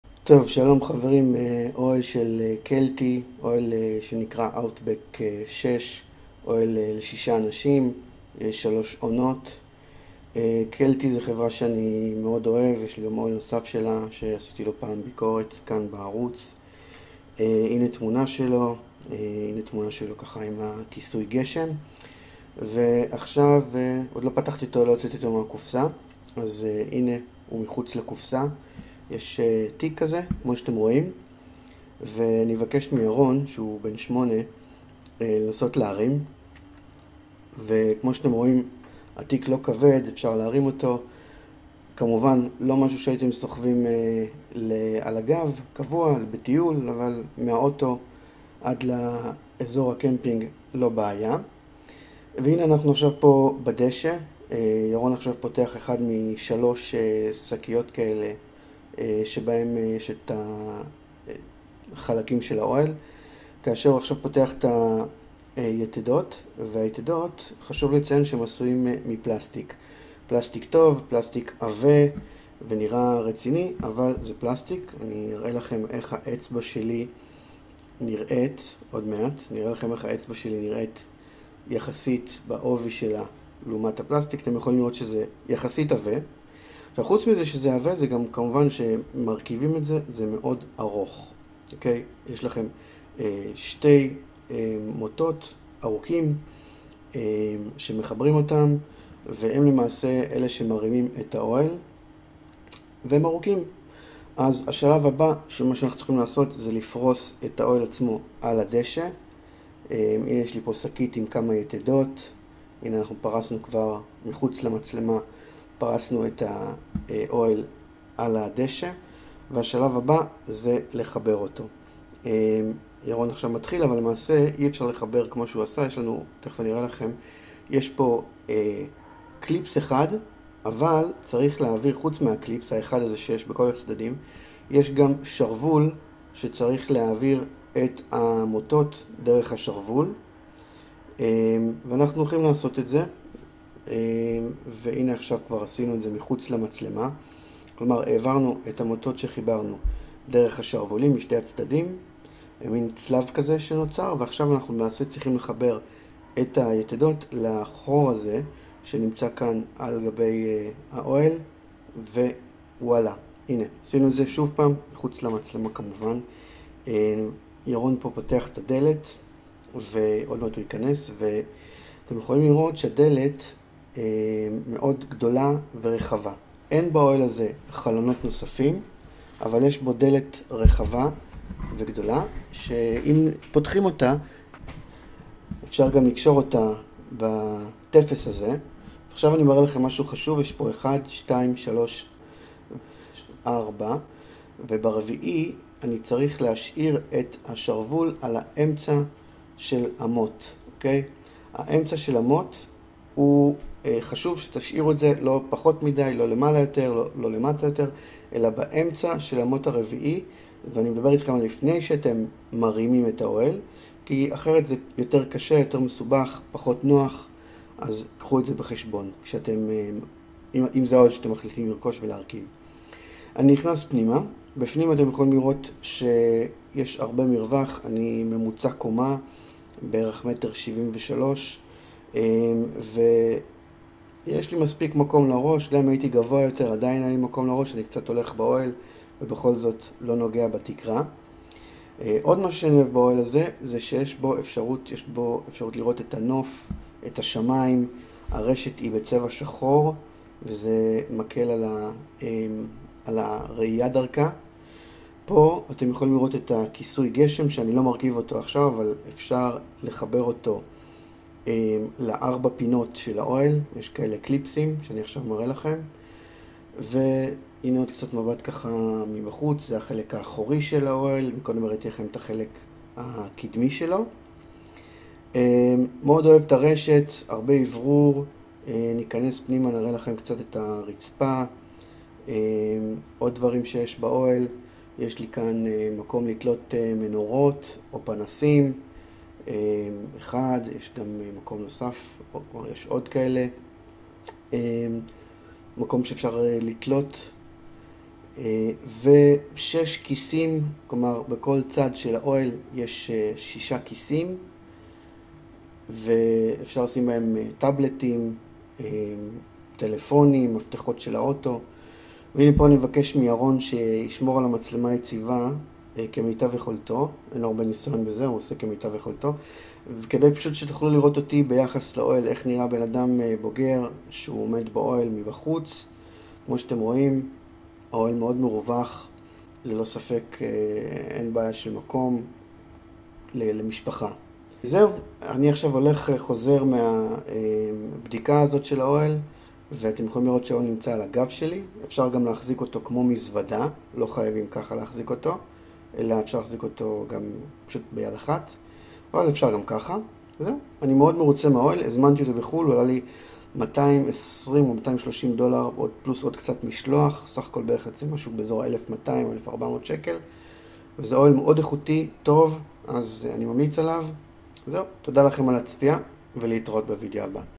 ביקורת אוהל שישה אנשים של חברת קלטי
את האוהל שבוידאו אני רכשתי באיביי והוא עלה כ 1400 ש"ח (כולל משלוח לישראל). לא זול אבל אוהל  איכותי ושימושי ואני אוהב את המוצרים של החברה הזאת.
שם הדגם למי שמתעניין:
kelty outback 6 person tent